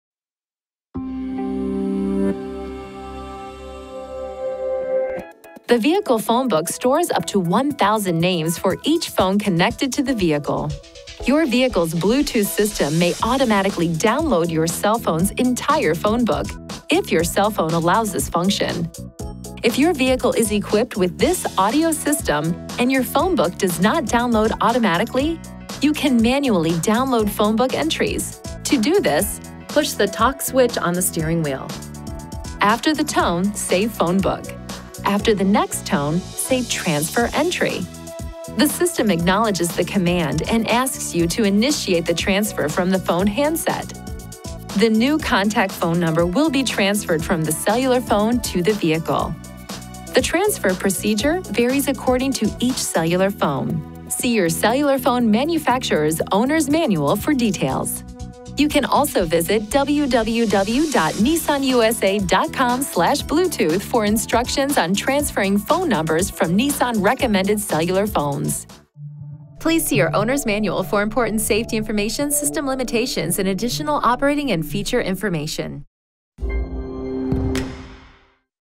2018 Nissan Altima - Vehicle Phonebook - without Navigation (if so equipped)
The vehicle phonebook stores up to 1,000 names for each phone connected to the vehicle. Your vehicle’s Bluetooth® system may automatically download your cell phone’s entire phonebook, if your cell phone allows this function.

If your vehicle is equipped with THIS audio system and your phonebook does not download automatically, you can manually download phonebook entries. To do this:
● Push the TALK switch on the steering wheel.
● After the tone, say “Phonebook”.
● After the next tone, say “Transfer Entry”.
● The system acknowledges the command and asks you to initiate the transfer from the phone handset.
● The new contact phone number will be transferred from the cellular phone to the vehicle.

The transfer procedure varies according to each cellular phone. See your cellular phone manufacturer’s Owner’s Manual for details.

for instructions on transferring phone numbers from NISSAN recommended cellular phones.